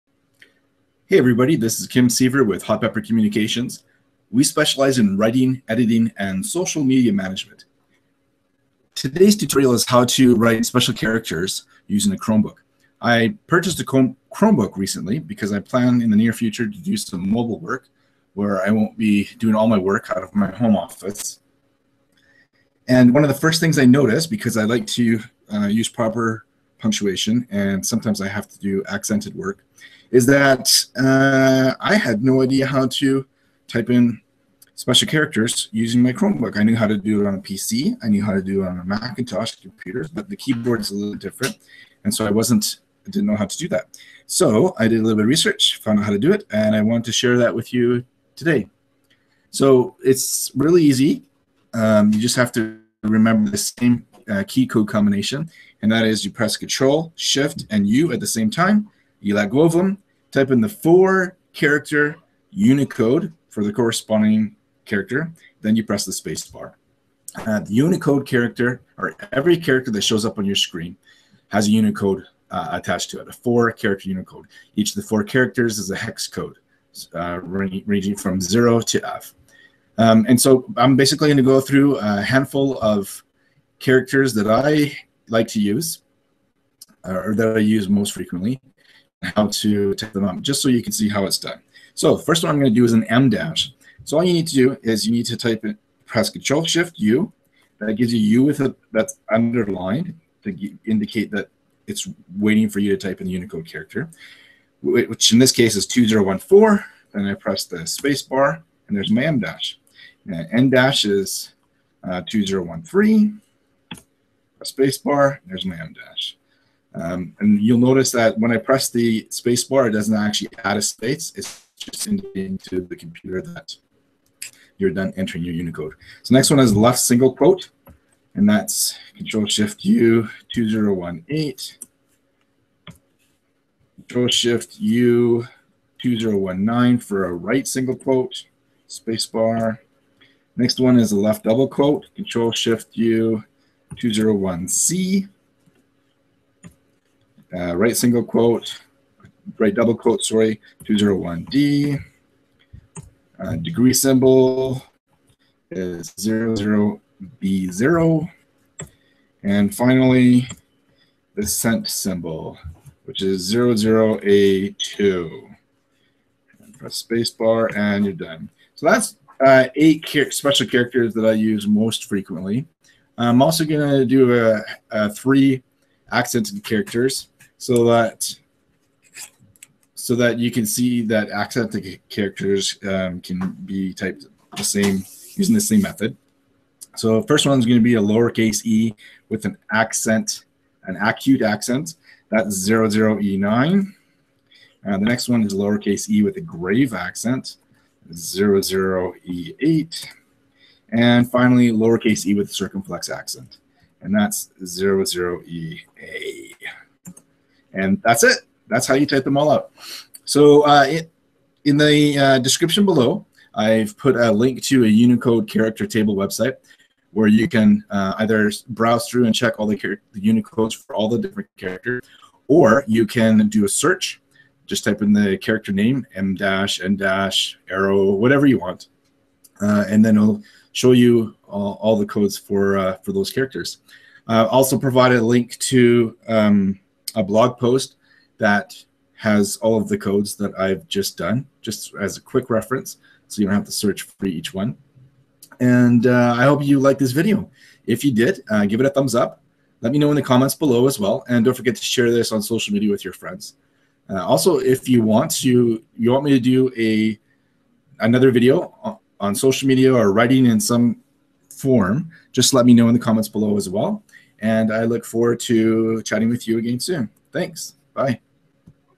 How to type special characters on your Chromebook
Social media and writing expert Kim Siever explains how to type special characters on a Chromebook